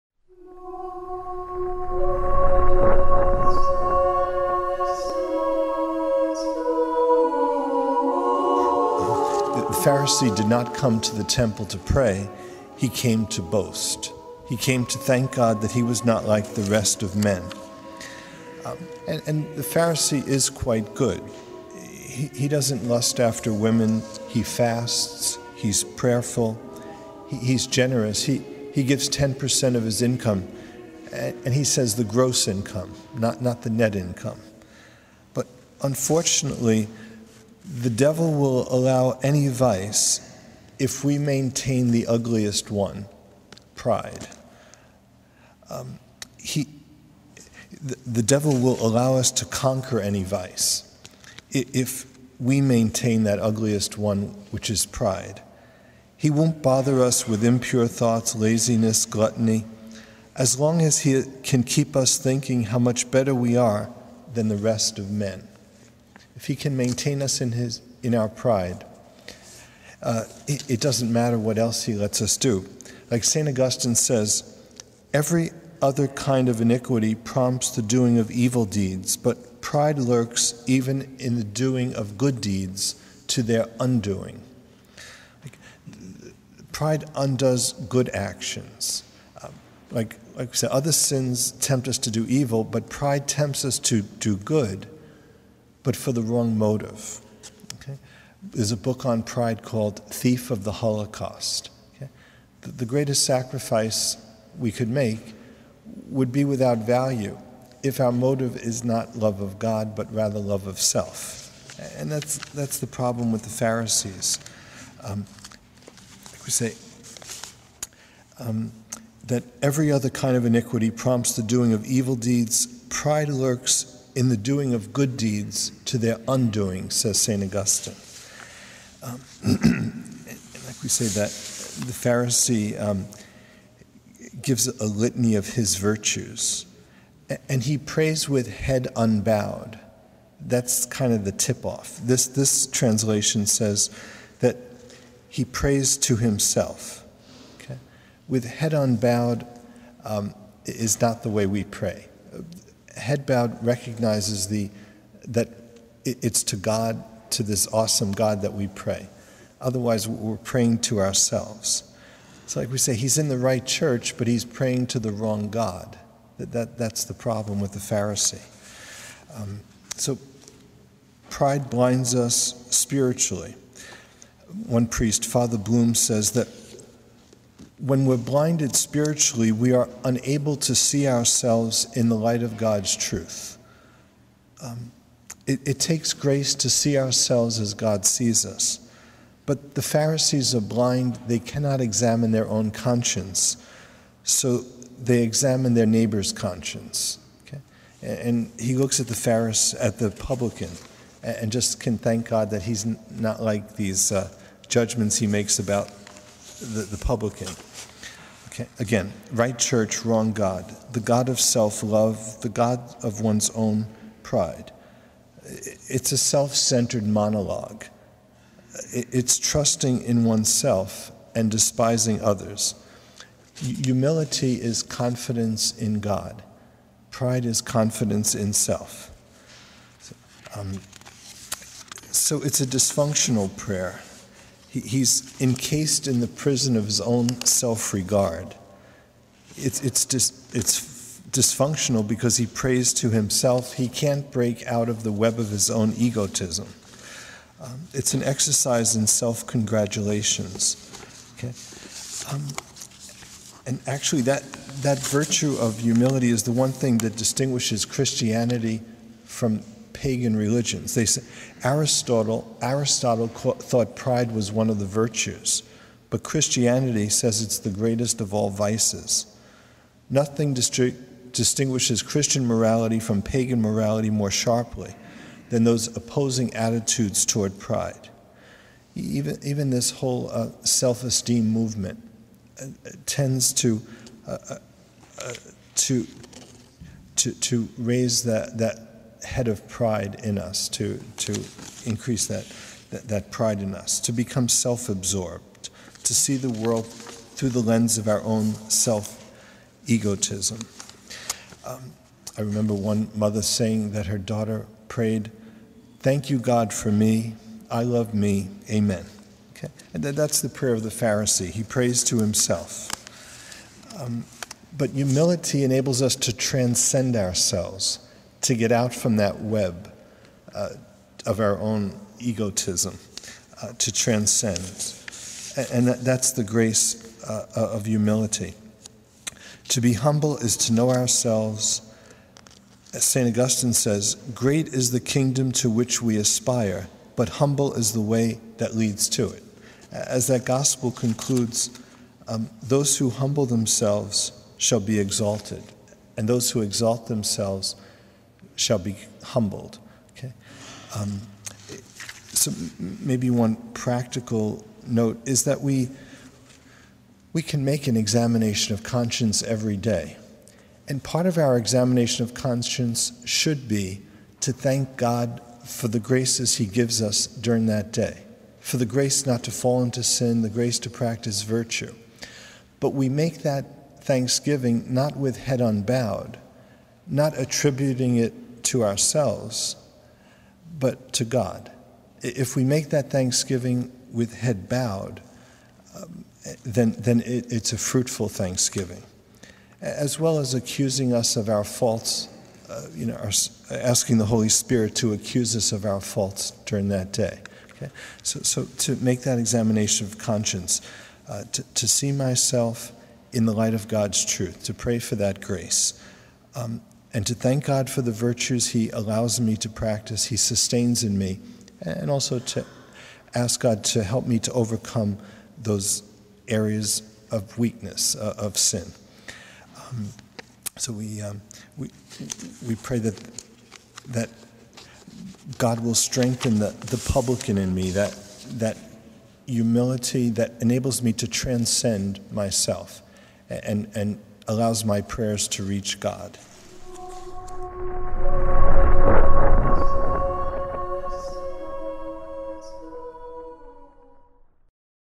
Fr Tito: Dysfunctional Prayer
Pride temps us to do good with the wrong motive. The pharisee prays to himself while the "sinner" bows his head and raises his pray to God.